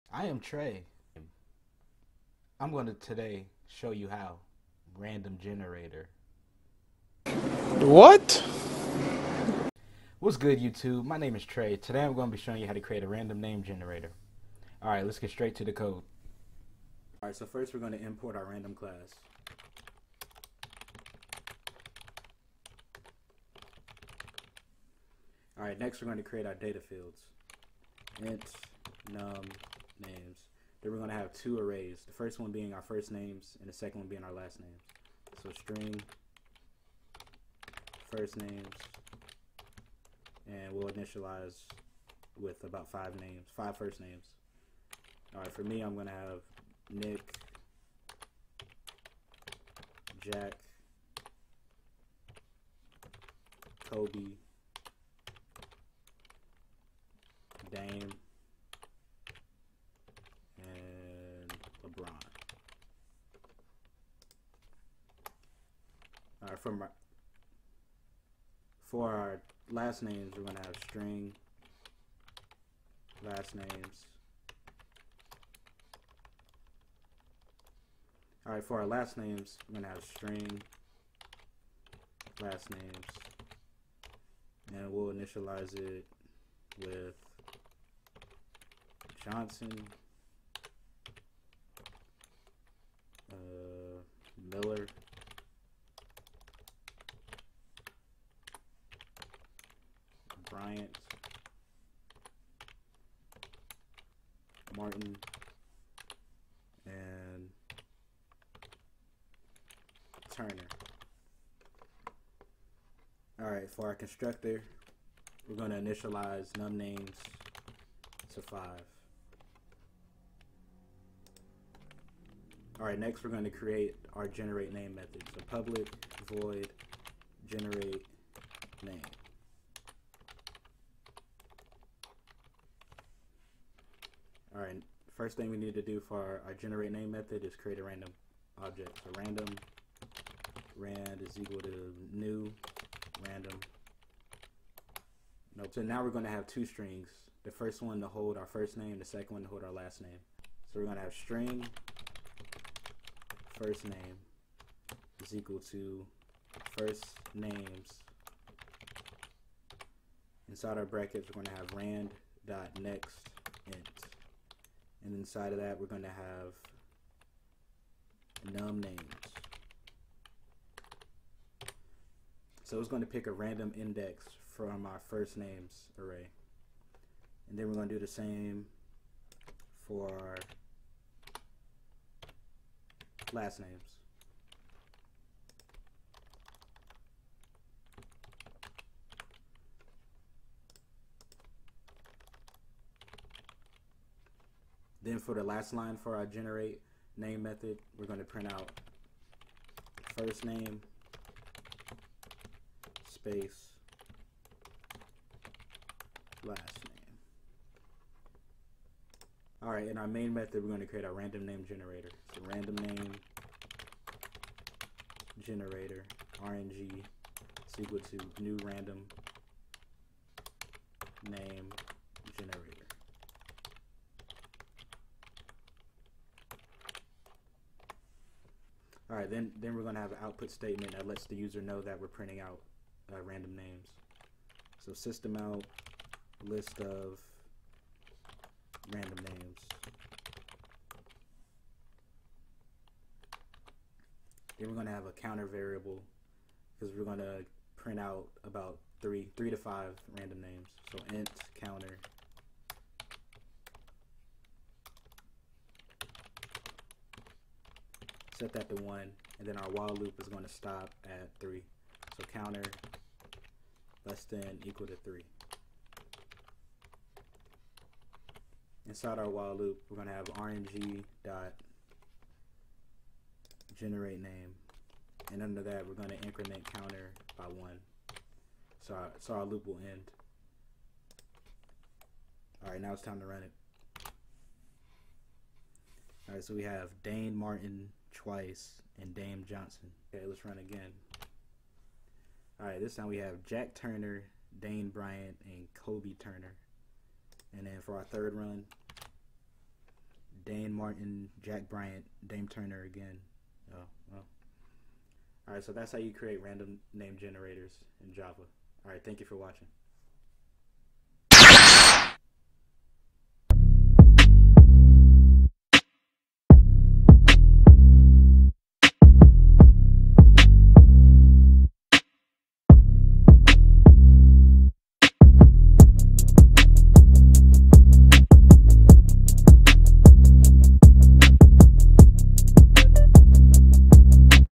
How to Generate Random Names in Java | Coding With Tre'
Today I am going to show you how to generate random names or strings
      in java. Enjoy the video!!! My bad for some of the editing mistakes.